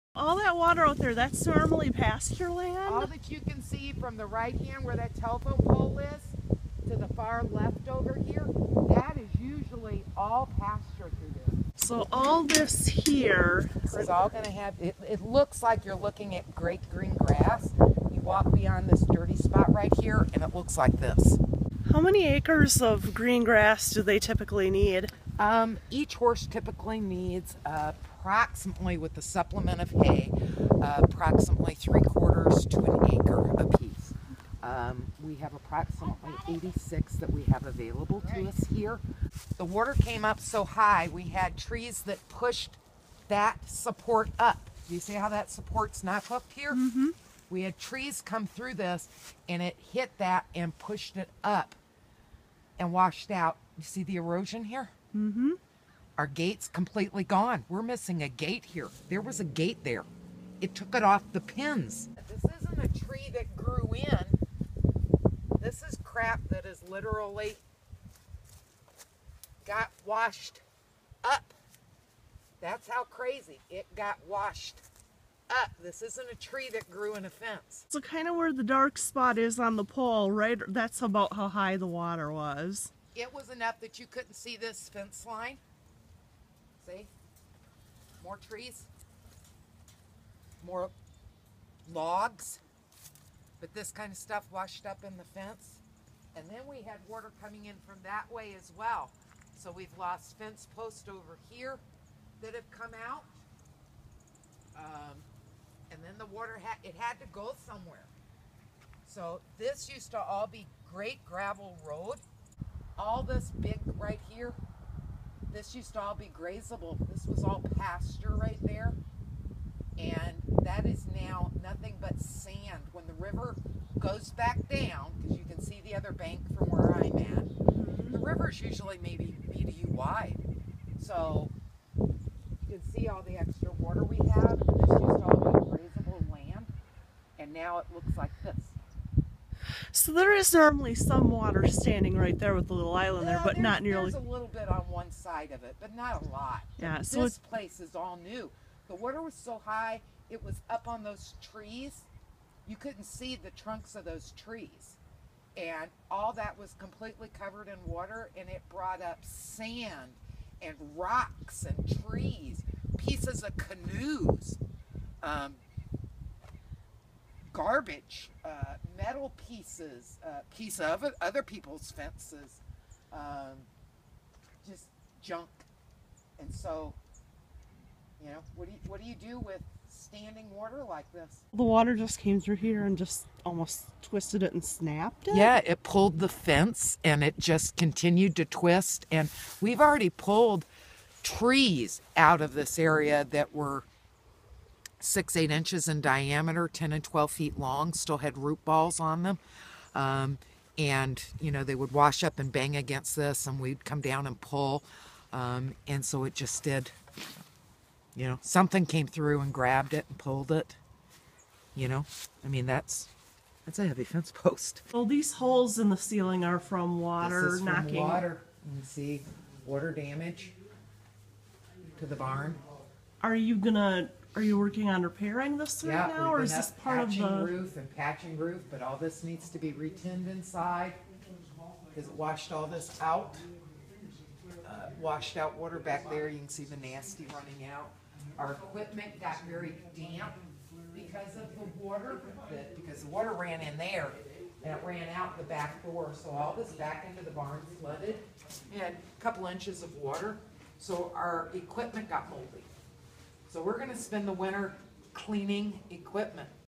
Flood damage at VETS in La Valle
Veterans Equine Trail Services in La Valle is recovering from September flooding that broke fence posts, carried away gates and left several acres of pasture land under mud and garbage. Barb Knopf, owner of VETS, showed a reporter the results of the flooding on Oct. 28, 2016. It had been rainy that week and water levels were high once again.